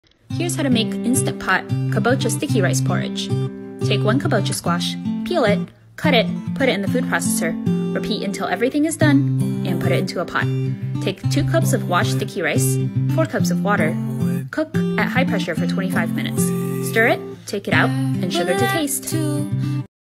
Easy Vegan Instant Pot Kabocha Squash Sticky Rice Porridge
Instant pot kabocha squash sticky rice porridge

This recipe is vegan and only requires 2 ingredients: kaboch and sticky rice!